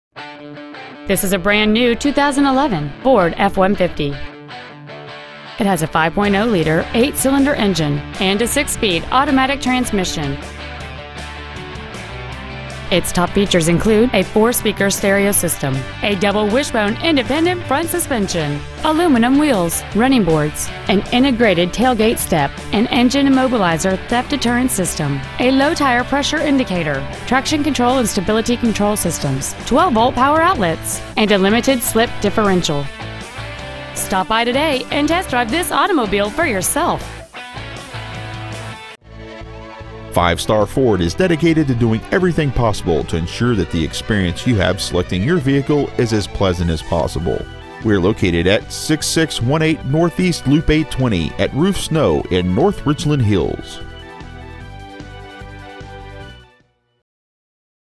2011 FORD F-150 TX
Year : 2011
 Make : FORD
 Model : F-150
 Engine : 5.0L V8
 Trans . : AUTO 6SPD
 Exterior : INGOT SILVER METALLIC
 Miles : 18
 Interior : BLACK
 Stock : BFA39724